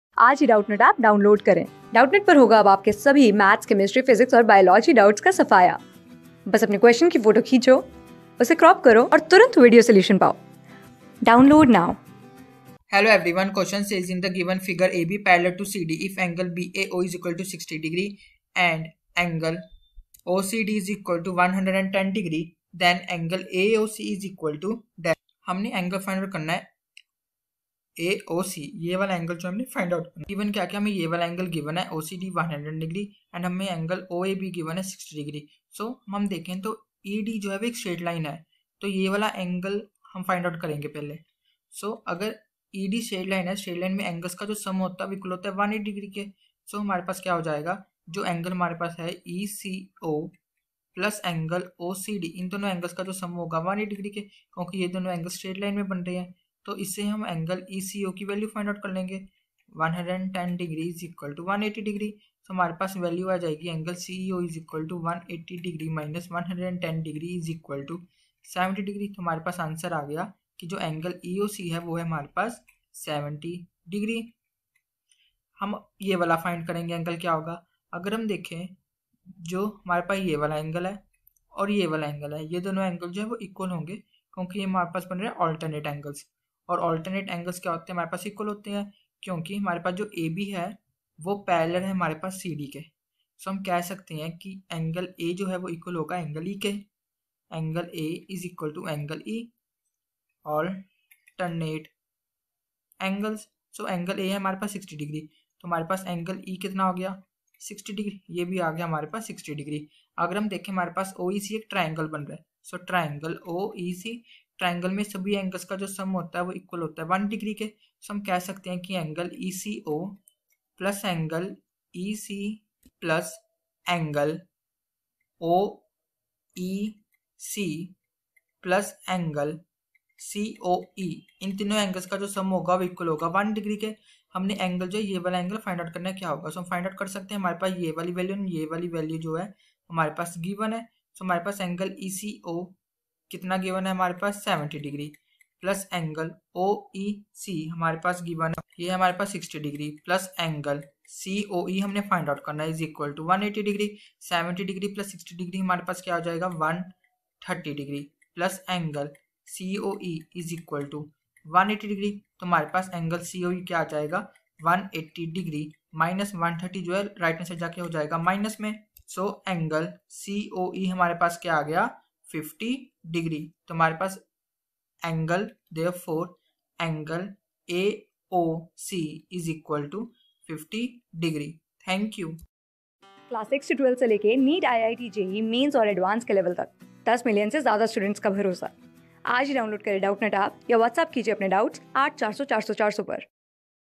In the given figure, AB||CD. If angleBAO=60^(@) and angleOCD=110^(@) then angleAOC=? | CLASS 9 ...
In the given figure, AB||CD. If angleBAO=60^(@) and angleOCD=110^(@) then angleAOC=?
Class: 9
Subject: MATHS
Chapter: LINES AND ANGLES
Board:CBSE

👉 Have Any Query? Ask Us.